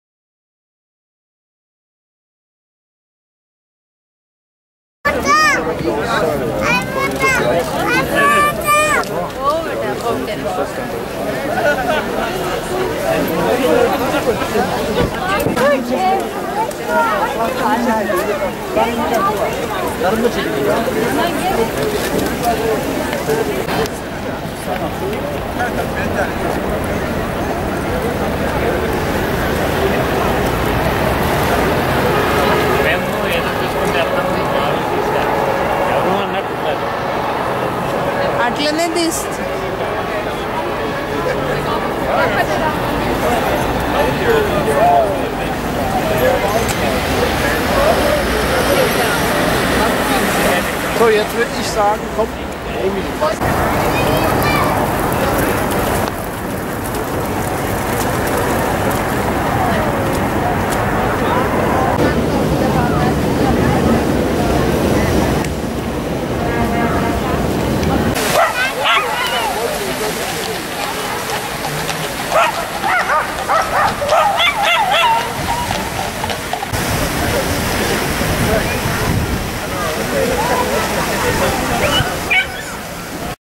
Brandenburg Gate, Berlin attractions, Germany
The Brandenburg Gate is a former city gate, rebuilt in the late 18th century as a neoclassical triumphal arch, and now one of the most well-known landmarks of Germany. Wikipedia
Opened: 1791
Address: Pariser Platz, 10117 Berlin, Germany
Architectural style: Greek Revival
Architect: Carl Gotthard Langhans